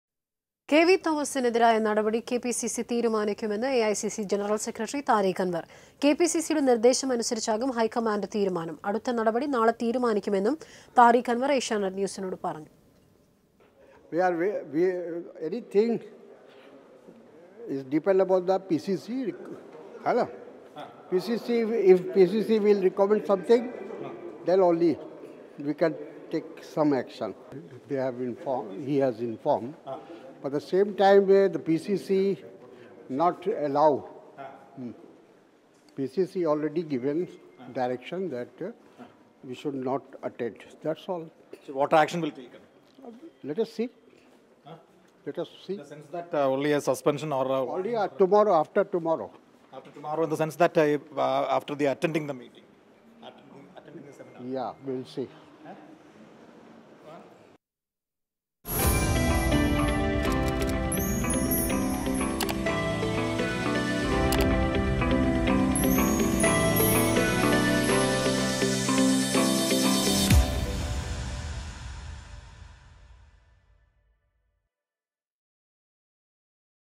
കെവി തോമസിനെതിരായ നടപടി കെപിസിസി തീരുമാനിക്കുമെന്ന് താരിഖ് അന്‍വര്‍ | Tariq Anwar
കെവി തോമസിനെതിരായ നടപടി കെപിസിസി തീരുമാനിക്കുമെന്ന് താരിഖ് അന്‍വര്‍. അടുത്ത നടപടി നാളെയെന്ന് അദ്ദേഹം ഏഷ്യാനെറ്റ് ന്യൂസിനോട്...
Congress leader Tariq Anwar about action against K V Thomas.